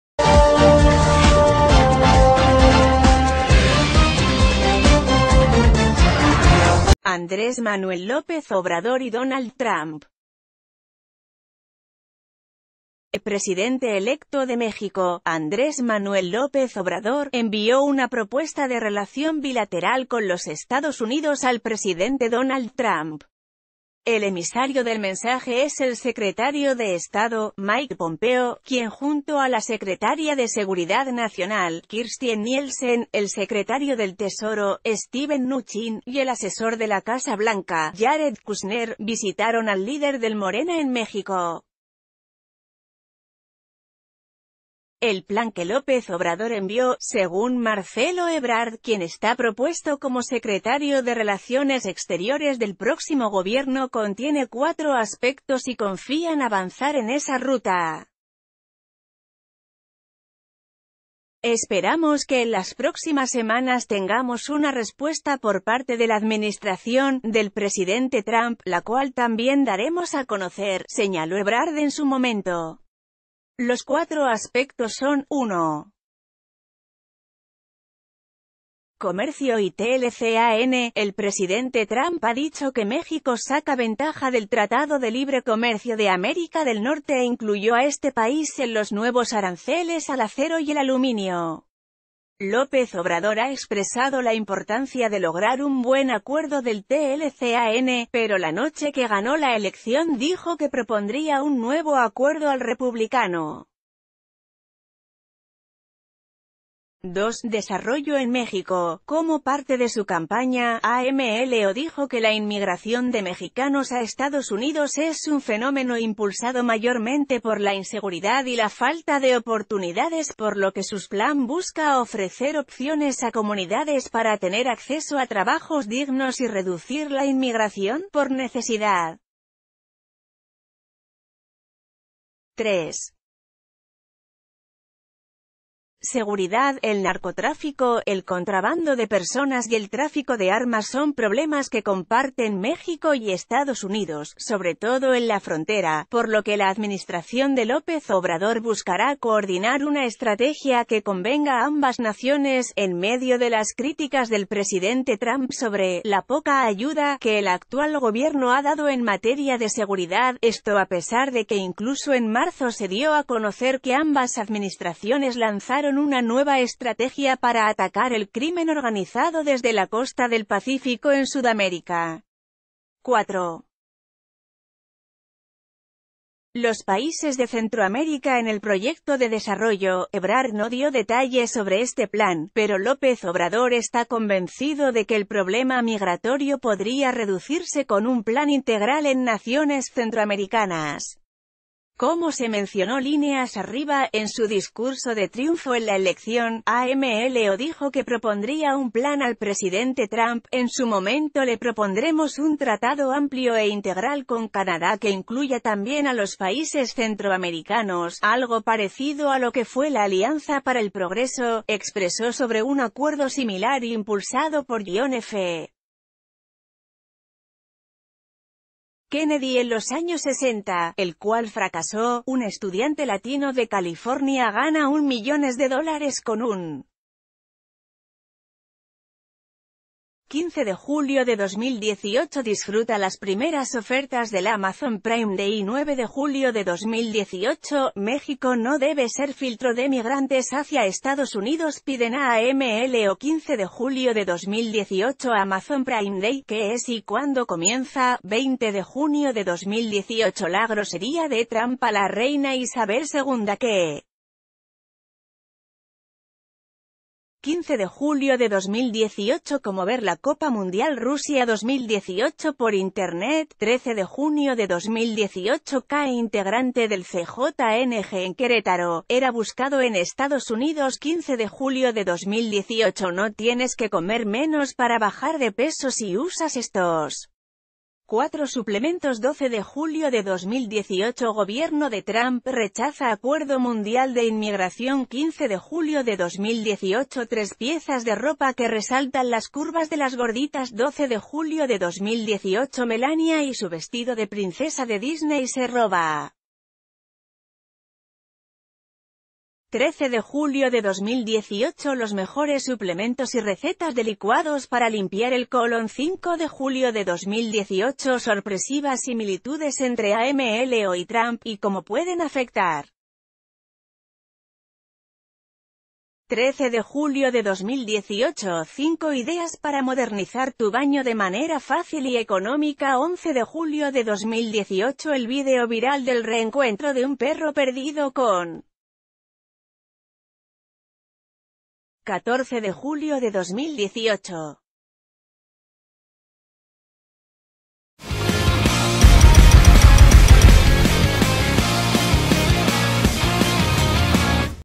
AMLO marca 4 temas prioritarios a Trump
Andrés Manuel López Obrador y Donald Trump. El presidente electo de México, Andrés Manuel López Obrador, envió una propuesta de relación bilateral con los Estados Unidos al presidente Donald Trump. El emisario del mensaje es el secretario de Estado, Mike Pompeo, quien junto a la secretaria de Seguri...